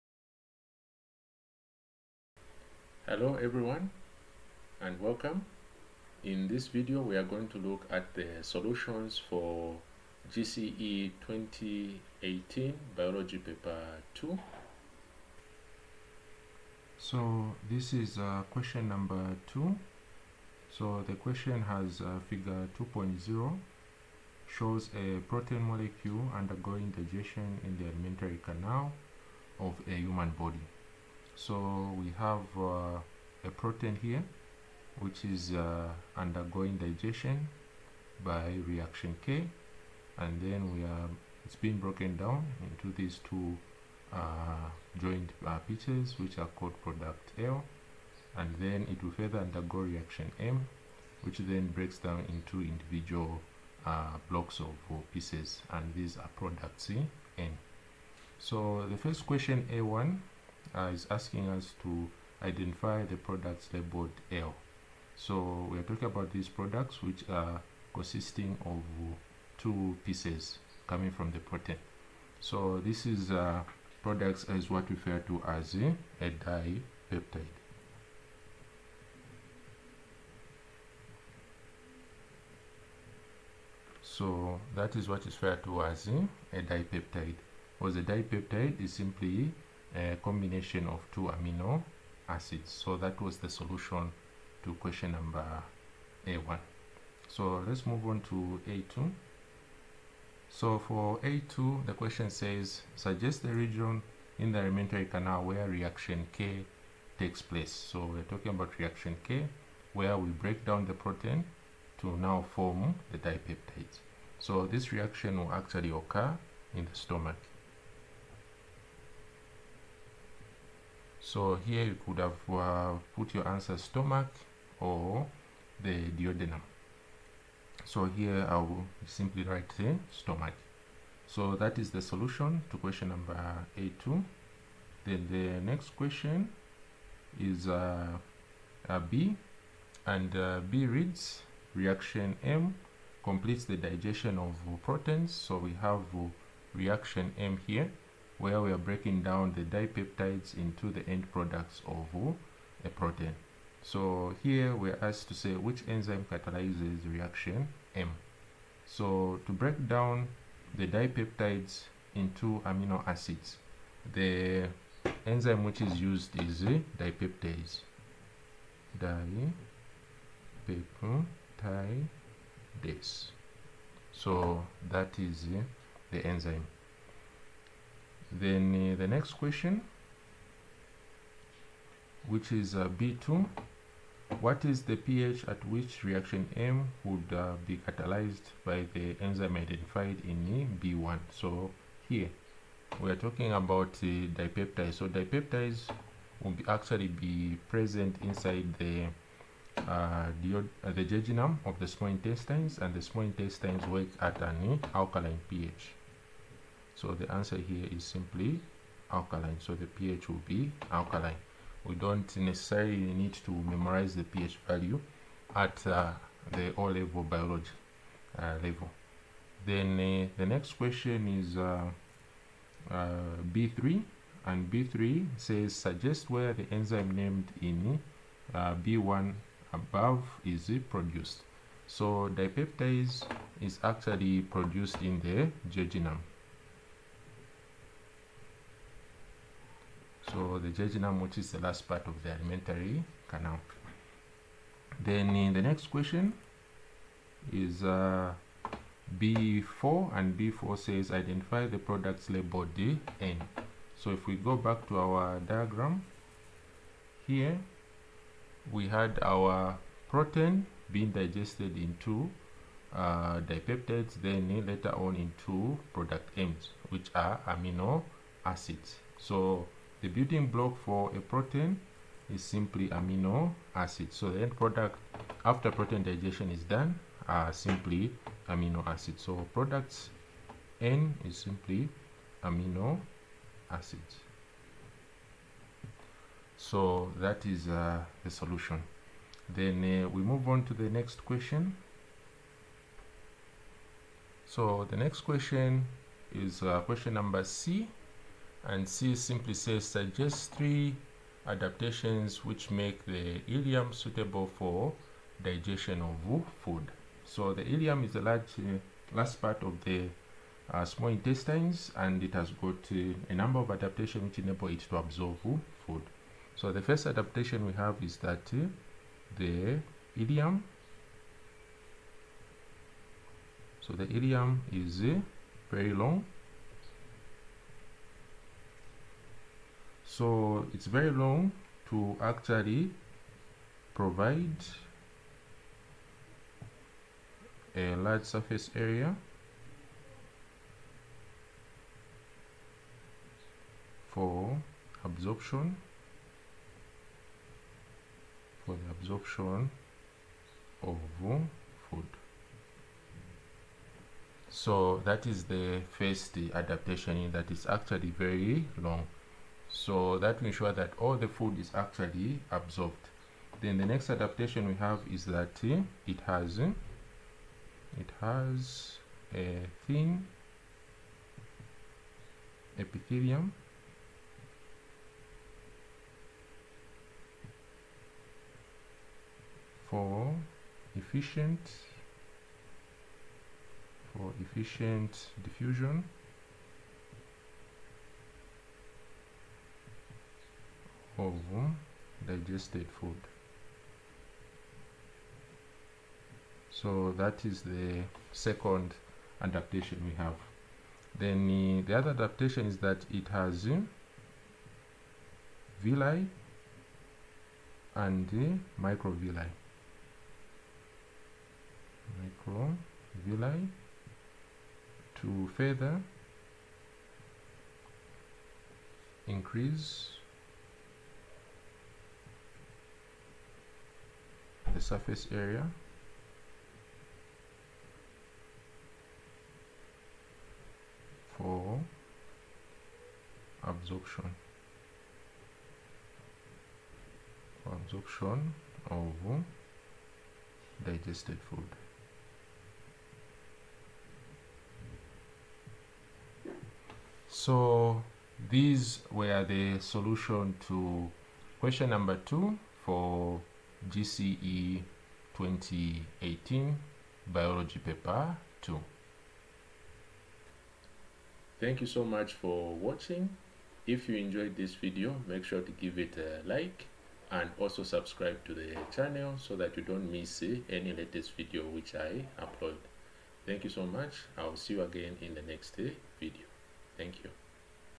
Biology paper 2 || GCE 2018 || Question 2 revision
Hello guys, In this video I will be describing the solutions to question 2 for  Biology paper 2 for GCE 2018 candidates for Examination council of Zambia.

- Sharing the video to your friends